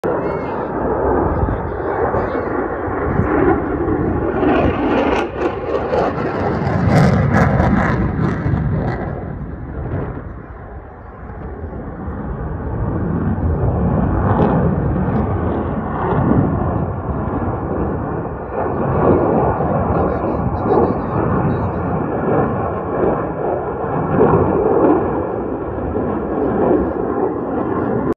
RAAF F-35 lightning 2 high speed pass and airobatics at the 2019 Edinburgh airshow
F-35 high speed pass